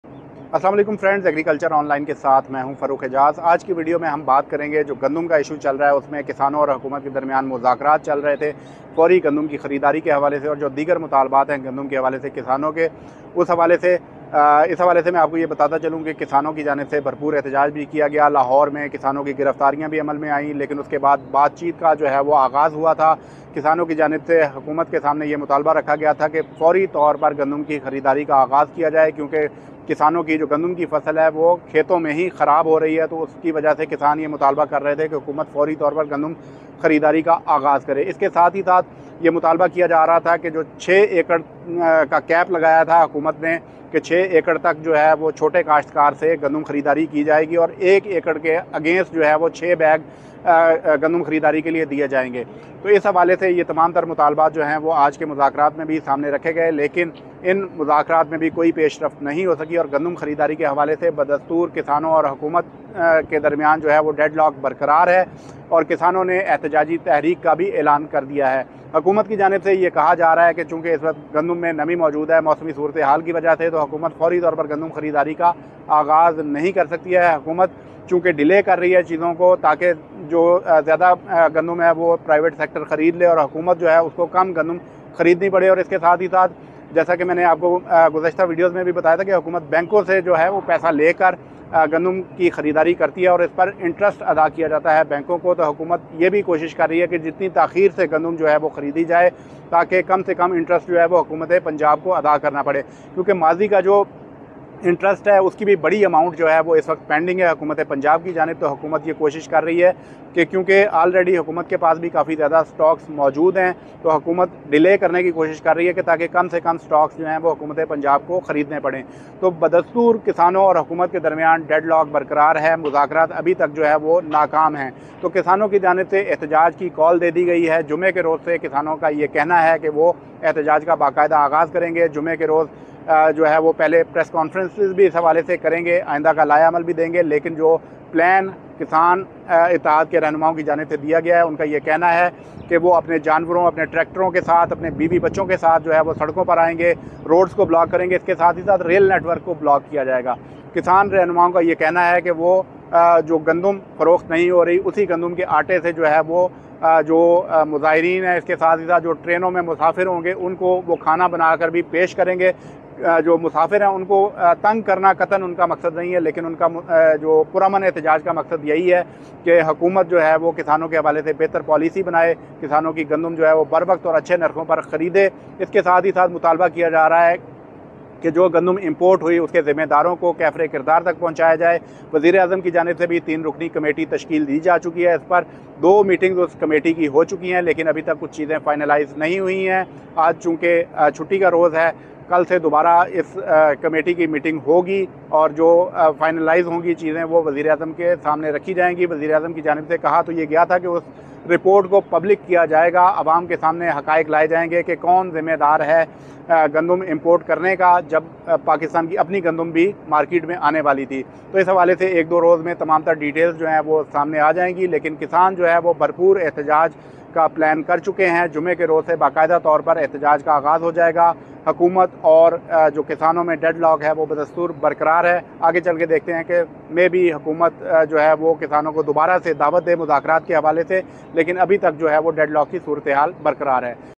گندم خریداری پر کسانوں اور حکومت کے مذاکرات ناکام
گندم خریداری پر کسانوں اور حکومت کے مذاکرات ناکام  ہو گئے